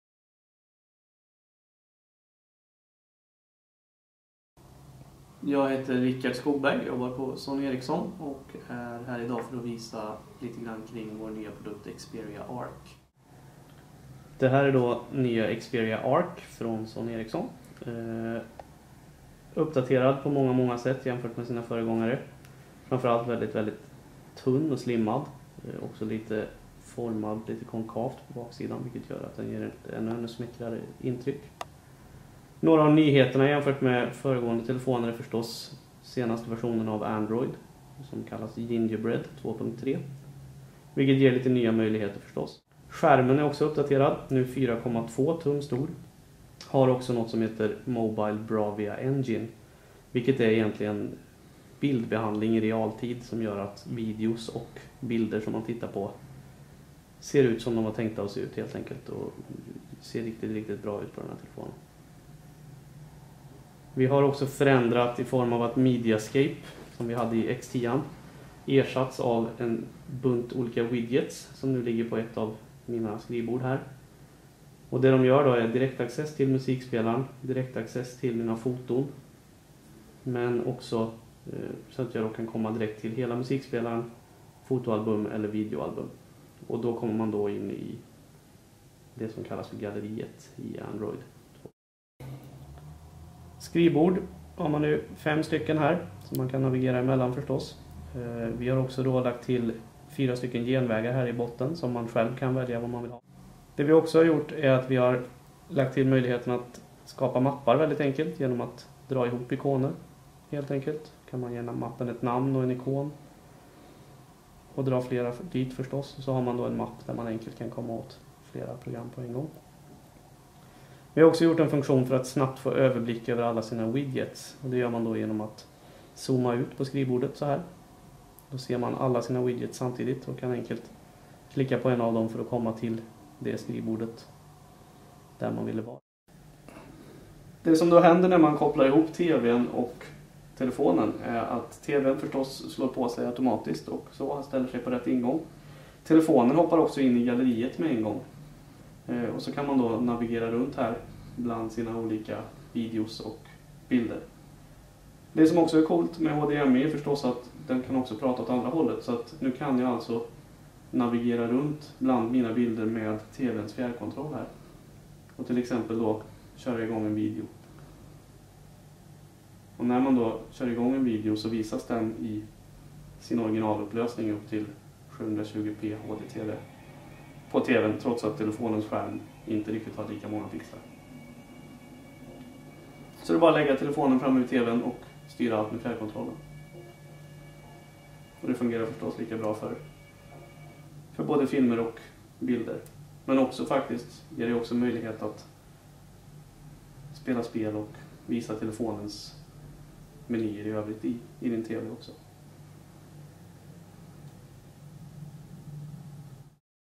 Sony Ericsson Xperia Arc
Sony Ericssons produktmarknadschef Rikard Skogberg visar sin nya androidmobil Xperia Arc.